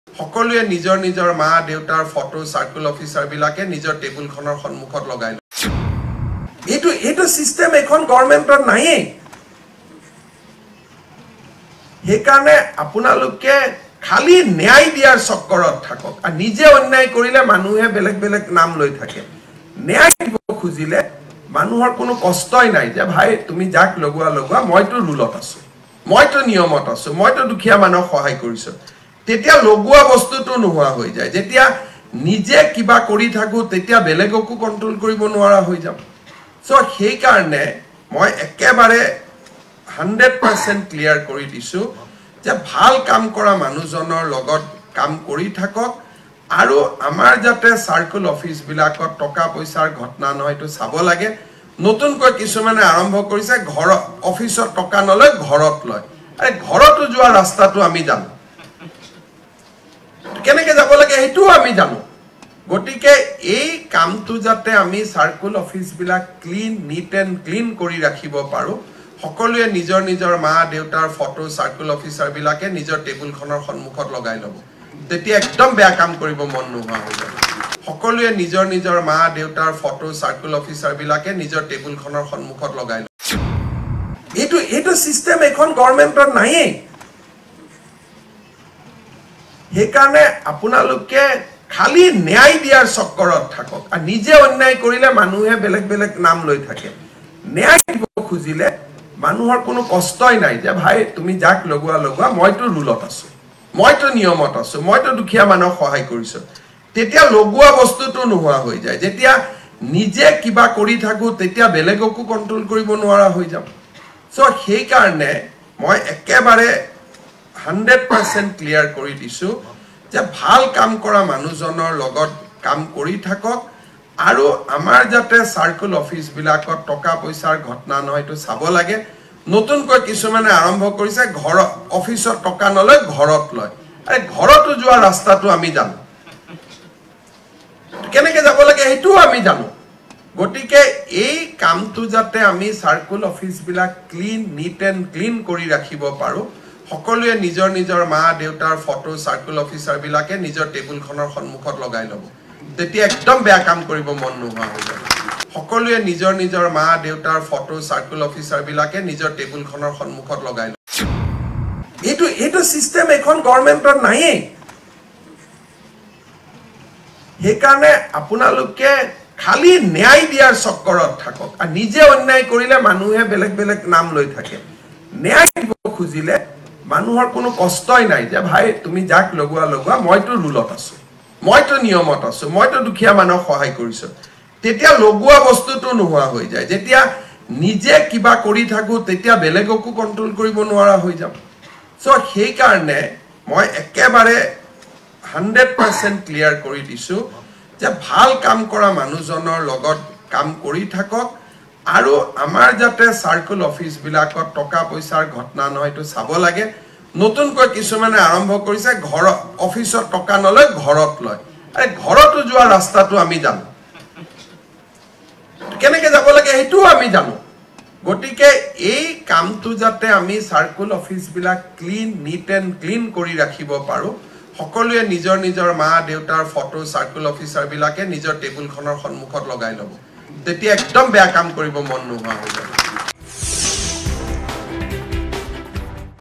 চাৰ্কোল অফিচাৰক নিজ নিজ পিতৃ-মাতৃৰ ফটো কাৰ্যালয়ত লগাবলৈ ক'লে মুখ্যমন্ত্ৰীয়ে।
চাৰ্কোল অফিচাৰক নিজ নিজ পিতৃ-মাতৃৰ ফটো কাৰ্যালয়ত লগাবলৈ ক'লে মুখ্যমন্ত্ৰীয়ে। পিতৃ-মাতৃৰ সন্মুখত বেয়া কাম কৰিব নোৱাৰিব।